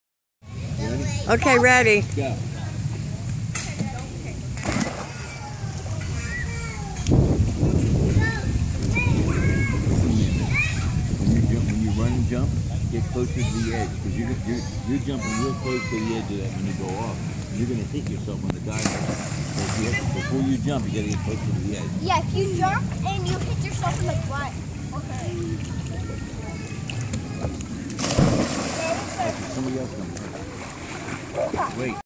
Kira and CJ with their cousin Jamie at the pool at Bakers Acres Campground in New Jersey!